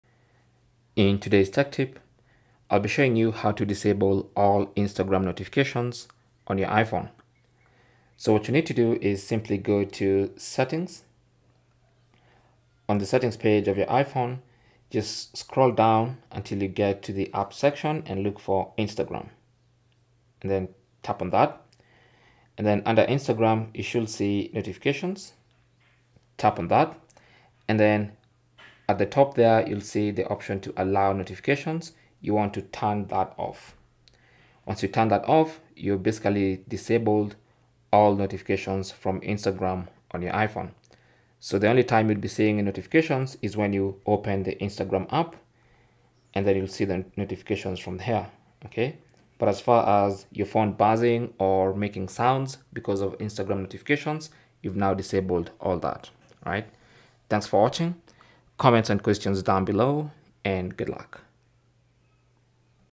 How To Disable Instagram Notifications On iPhone
In this video, we'll walk you through the simple process to disable Instagram notifications on your iPhone.

Are you tired of being constantly bombarded with notifications from Instagram on your iPhone? Whether it's likes, comments, or direct messages, sometimes it can feel like too much.

In this step-by-step tutorial, we'll show you exactly how to take control and turn off those pesky notifications once and for all.

Disable Instagram Notifications On iPhone:
How To Turn OFF Instagram Notifications On iPhone:
How To Deactivate Instagram Notifications On iPhone:

Step 1: Begin by unlocking your iPhone and navigating to the Settings app. This can typically be found on your home screen, represented by a gear icon.

Step 2: Once in the Settings app, scroll down and tap on "Instagram". Tap on it to access the notification settings specific to the Instagram app.

Step 3: Under Instagram, tap on the "Notifications" option. Within the Instagram notification settings, you'll find various options to customize how notifications are displayed. The "Allow Notifications" option should be toggled on.

Step 4: Tap on the "Allow Notifications" to turn the Instagram notifications off. Now, you will not be able to receive these notifications on your iPhone. You can only see and interact with your notifications once you open the Instagram app.

Cell Phone Tripod Adapter